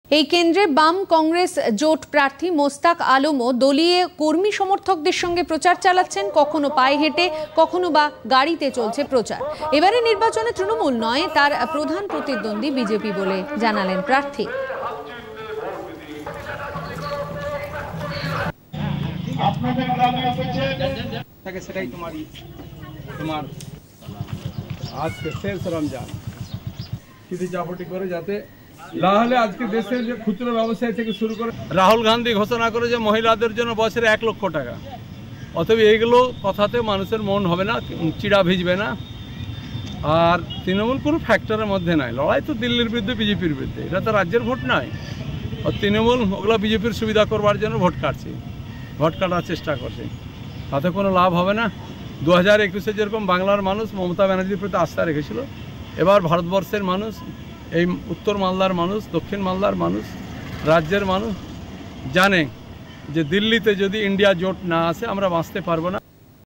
নির্বাচনী প্রচারে উত্তর মালদা লোকসভা কেন্দ্রের বাম-কংগ্রেস জোট প্রার্থী
নির্বাচনী প্রচারে উত্তর মালদা লোকসভা কেন্দ্রের বাম-কংগ্রেস জোট প্রার্থী মোস্তাক আলম।